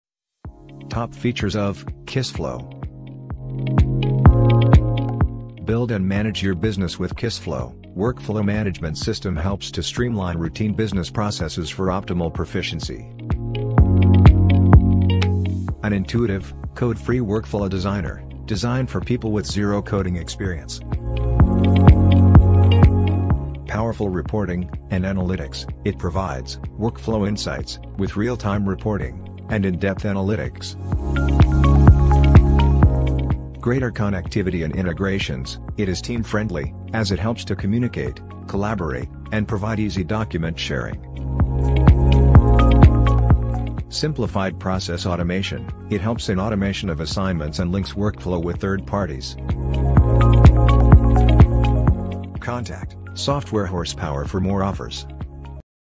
Top Features of Kissflow | Kissflow Workflow Tutorial - Software Horsepower
Build and Manage your Business with Kiss flow
An Intuitive, Code-Free Workflow Designer
Powerful Reporting and Analytics
Simplified Process Automation
Greater Connectivity & Integrations

🔑Key Features:-

✅ How Can Kissflow Enhance Your Enterprise Workflow Management?

 ➡️No-code tool
 ➡️Easy integration
 ➡️Secure data
 ➡️Scalable
 ➡️Anytime anywhere access

✅ What is a workflow management system?

✅ The internet's most powerful no-code tools

 ➡️Visual Form and Workflow Designer
 ➡️SLA and Escalations
 ➡️Automated Workflow Routing
 ➡️Algorithmic Task Assignment
 ➡️Third-Party Integrations
 ➡️Ticketing System
 ➡️Contextual Collaboration
 ➡️Reporting & Analytics

✅ Features every workflow management system should have

 ➡️WYSIWYG form designer
 ➡️Integration with other cloud apps
 ➡️Cloud-based technology
 ➡️KPI-based reports
 ➡️SLA status indicators

👉 Recommended Other Softwares